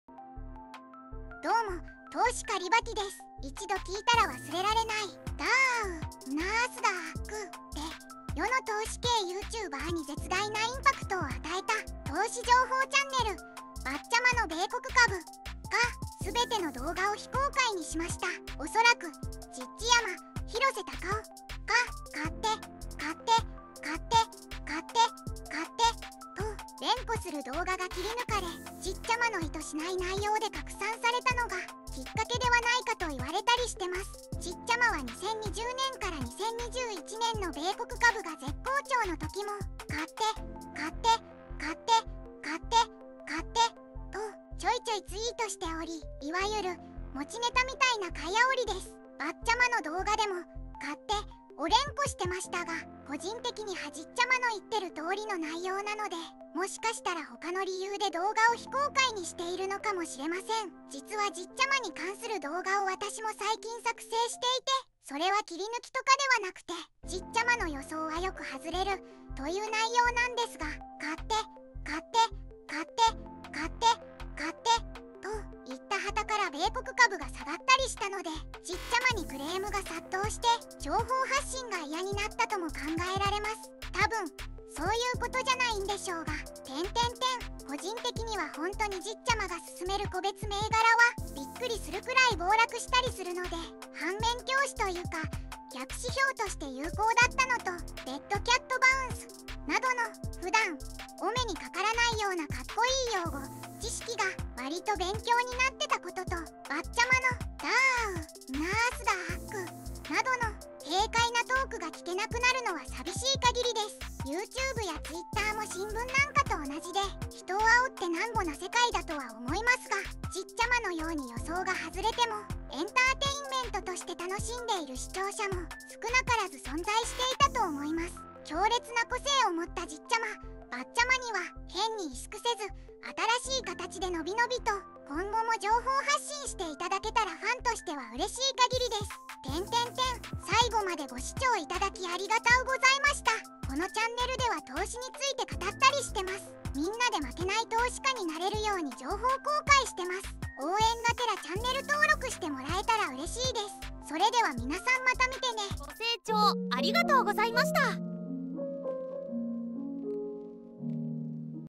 消えた「ばっちゃまの米国株」！じっちゃま～
どうも、投資家リバティです
一度聞いたら忘れられない
「ダァァウゥ」
「ﾝナァアスダァァックゥ」
で、世の投資系YOUTUBERに絶大なインパクトを与えた
投資情報チャンネル「ばっちゃまの米国株」が
全ての動画を非公開にしました
恐らく、じっちゃま（広瀬隆雄）が
「買って！買って！買って！買って！買って！」と
連呼する動画が切り抜かれ
じっちゃまの意図しない内容で拡散されたのが
切っ掛けではないかと言われたりしてます
じっちゃまは2020年～2021年の米国株が絶好調の時も
ちょいちょいツイートしており
所謂、持ちネタみたいな買い煽りです
ばっちゃまの動画でも「買って！」を連呼してましたが
個人的にはじっちゃまの言ってる通りの内容なので
もしかしたら他の理由で動画を非公開にしているのかもしれません
実はじっちゃまに関する動画を私も最近作成していて
それは切り抜きとかではなくて
「じっちゃまの予想はよく外れる」という内容なんですが
いった傍から米国株が下がったりしたので
じっちゃまにクレームが殺到して
情報発信が嫌になったとも考えられます
多分、そういうことじゃないんでしょうが。。
個人的にはホントにじっちゃまが薦める個別銘柄は
ビックリするくらい暴落したりするので
反面教師というか、逆指標として有効だったのと、
「デッドキャットバウンス」などの
普段、お目に掛からないようなカッコいい用語・知識が
割と勉強になってたことと、
ばっちゃまの
「ダァァウゥ」
「ﾝナァアスダァァックゥ」などの
軽快なトークが聞けなくなるのは寂しい限りです
人を煽ってナンボな世界だとは思いますが
じっちゃまのように予想が外れても
エンターテインメントとして楽しんでいる視聴者も
少なからず存在していたと思います
強烈な個性を持ったじっちゃま・ばっちゃまには
変に委縮せず、新しい形でのびのびと
今後も情報発信して頂けたらファンとしては嬉しい限りです！
最後までご視聴いただき有難うございました
このチャンネルでは投資について語ったりしてます
みんなで負けない投資家になれるように情報公開してます
それではみなさんまた見てね

【投資法】
NISAでSP500をコアに積立投資してます😄